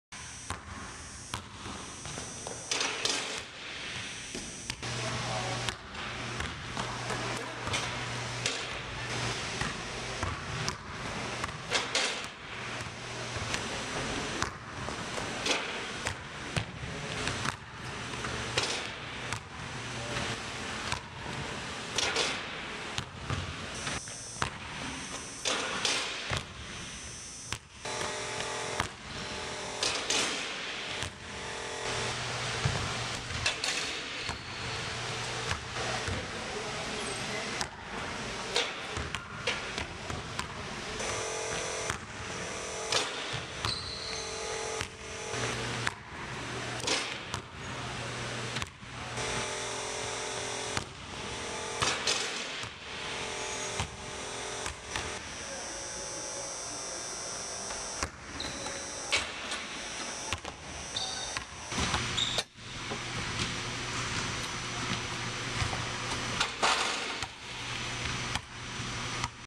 contest dunks
dunks from my dunk contest with no slow motion or music, so you can hear the rim sing  :)